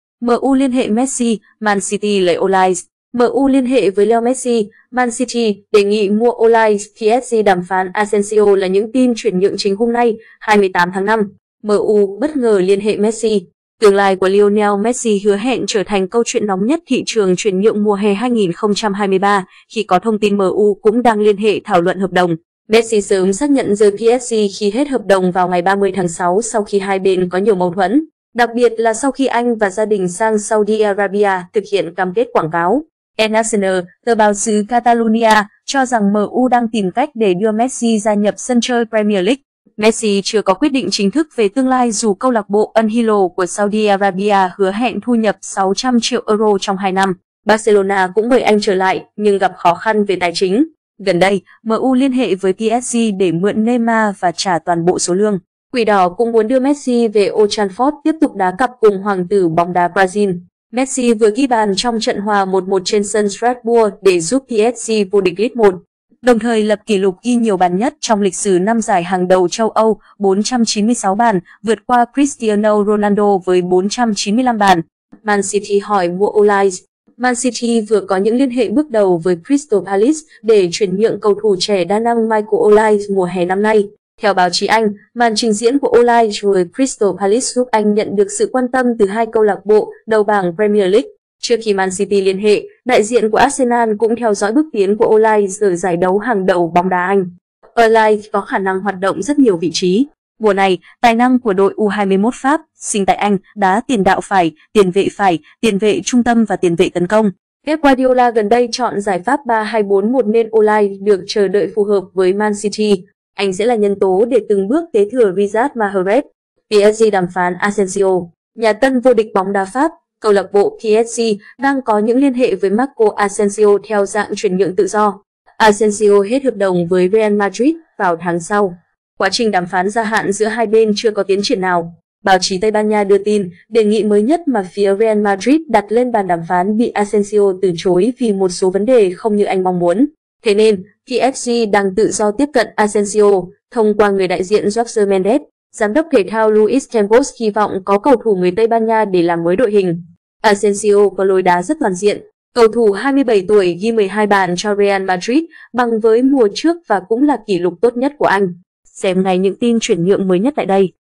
MU liên hệ Messi, Man City lấy Olise
MU liên hệ với Leo Messi, Man City đề nghị mua Olise, PSG đàm phán Asensio là những tin chuyển nhượng chính hôm nay, 28/5.Tương lai của Lionel Messi hứa hẹn trở thành câu chuyện nóng nhất thị trường chuyển nhượng mùa Hè 2023, khi có thông tin MU cũng đang liên hệ thảo luận hợp đồng.
"Quỷ đỏ" cũng muốn đưa Messi về Old Trafford tiếp tục đá cặp cùng "Hoàng tử bóng đá Brazil".
El Nacional, tờ báo xứ Catalunya, cho rằng MU đang tìm cách để đưa Messi gia nhập sân chơi Premier League.
Asensio có lối đá rất toàn diện. Cầu thủ 27 tuổi ghi 12 bàn cho Real Madrid, bằng với mùa trước và cũng là kỷ lục tốt nhất của anh.
Messi vừa ghi bàn trong trận hòa 1-1 trên sân Strasbourg để giúp PSG vô địch Ligue 1, đồng thời lập kỷ lục ghi nhiều bàn nhất trong lịch sử 5 giải hàng đầu châu Âu (496 bàn, vượt qua Cristiano Ronaldo với 495 bàn).
Theo báo chí Anh, màn trình diễn của Olise với Crystal Palace giúp anh nhận được sự quan tâm từ hai CLB đầu bảng Premier League.
Messi chưa có quyết định chính thức về tương lai dù CLB Al-Hilal của Saudi Arabia hứa hẹn thu nhập 600 triệu euro trong hai năm. Barcelona cũng mời anh trở lại nhưng gặp khó khăn về tài chính.
Olise là mục tiêu của Man City
Xem ngay nhữngtin chuyển nhượngmới nhất tại đây!
Olise có khả năng hoạt động rất nhiều vị trí. Mùa này, tài năng của đội U21 Pháp (sinh tại Anh) đá tiền đạo phải, tiền vệ phải, tiền vệ trung tâm và tiền vệ tấn công.
Asensio hết hợp đồng với Real Madrid vào tháng sau. Quá trình đàm phán gia hạn giữa hai bên chưa có tiến triển nào.
Báo chí Tây Ban Nha đưa tin, đề nghị mới nhất mà phía Real Madrid đặt lên bàn đàm phán bị Asensio từ chối vì một số vấn đề không như anh mong muốn.
PSG muốn có Asensio
Pep Guardiola gần đây chọn giải pháp 3-2-4-1 nên Olise được chờ đợi phù hợp với Man City. Anh sẽ là nhân tố để từng bước kế thừa Riyad Mahrez.
Thế nên, PSG đang tự do tiếp cận Asensio, thông qua người đại diện Jorge Mendes. GĐTT Luis Campos hy vọng có cầu thủ người Tây Ban Nha để làm mới đội hình.
PSG đàm phán Asensio
Messi vừa ghi bàn giúp PSG vô địch Ligue 1
Messi sớm xác nhận rời PSG khi hết hợp đồng vào 30/6 sau khi hai bên có nhiều mâu thuẫn, đặc biệt là sau khi anh và gia đình sang Saudi Arabia thực hiện cam kết quảng cáo.
Man City hỏi mua Olise